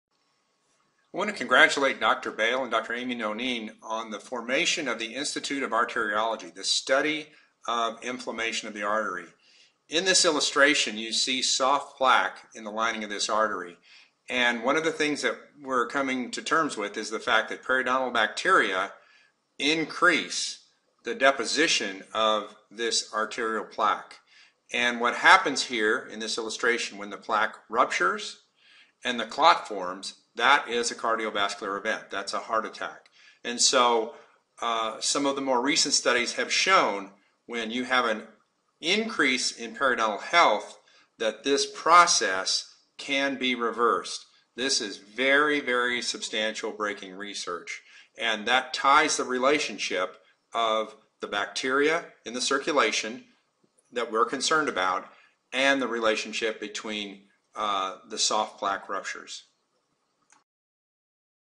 Coronary Soft Plaque Rupture
A Coronary Soft Plaque Rupture is shown